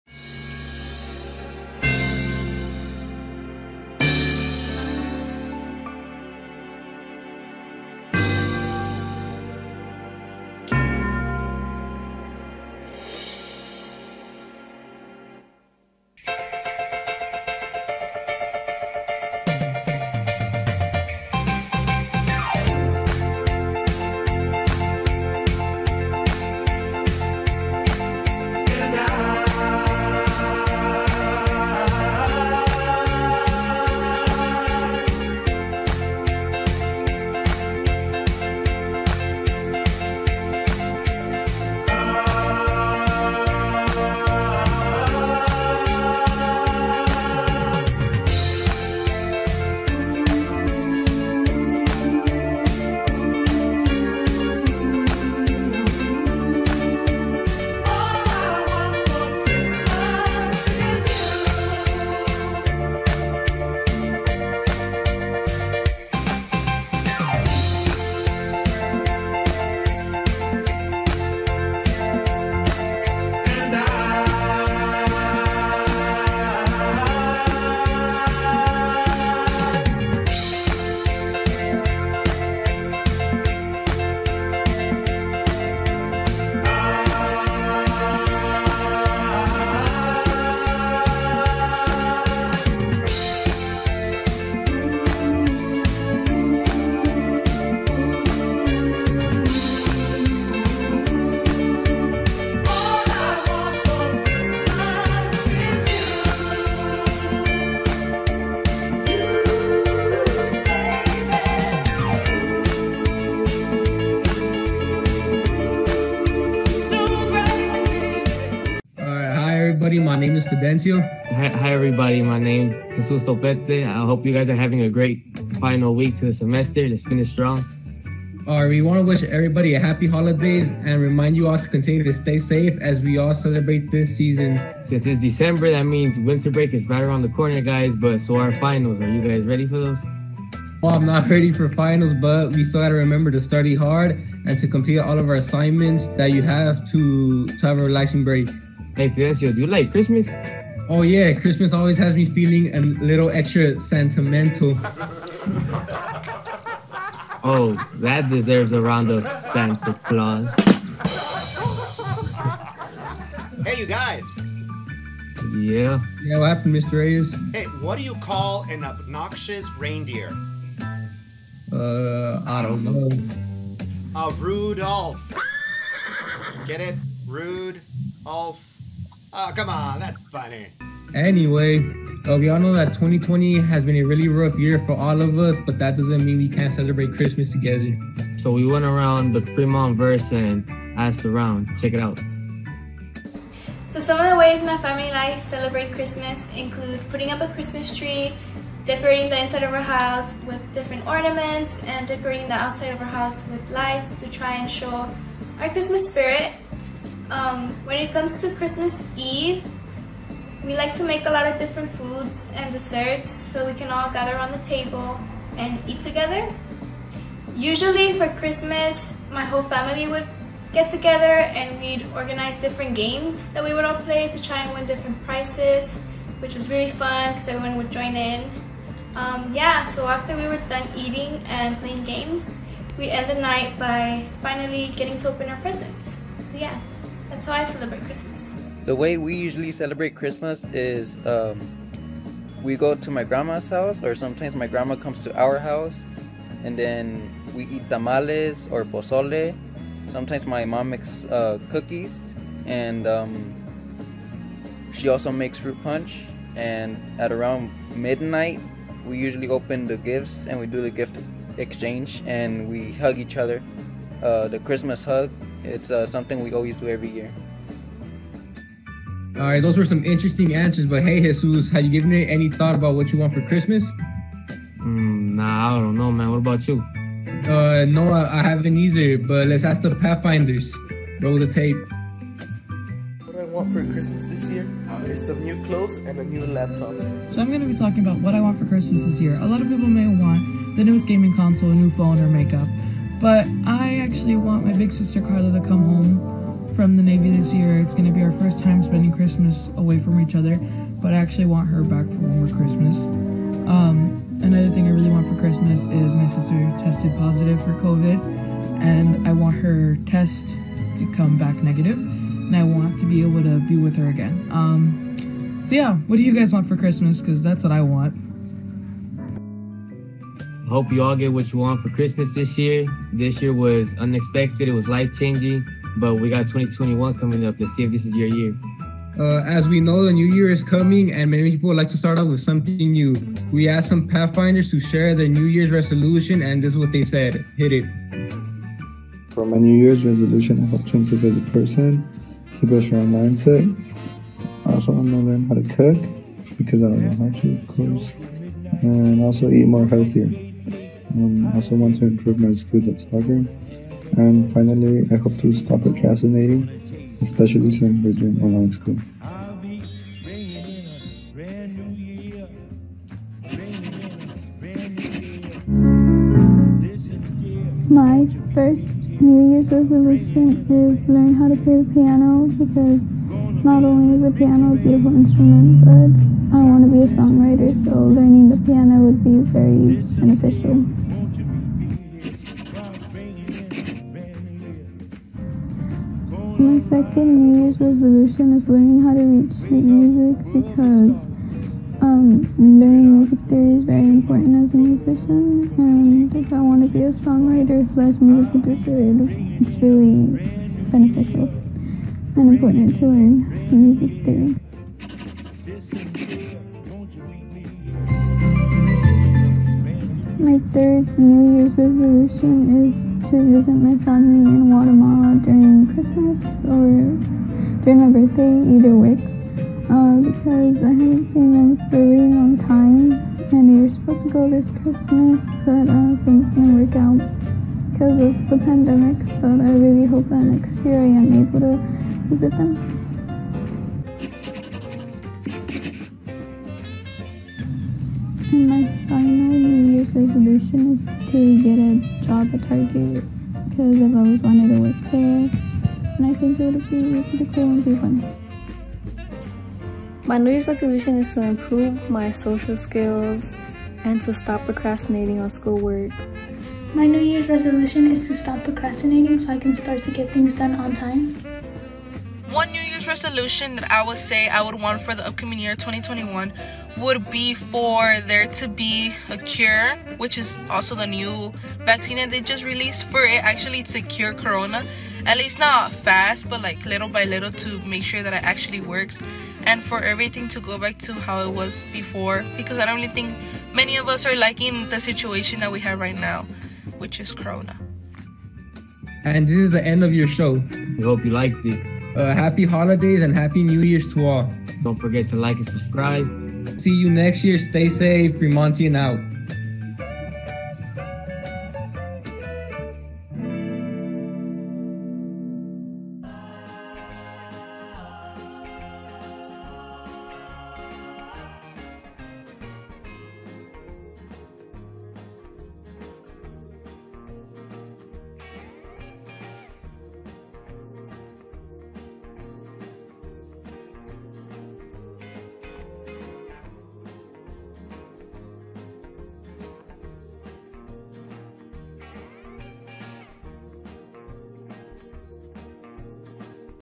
Fremontian The Shutdown Edition ep 3 HAPPY HOLIDAYS!
This is our last Fremontian of the year! The team went around asking for holiday traditions and New Year's resolutions. Happy Holidays and New Years to all of you! We will see you all in 2021!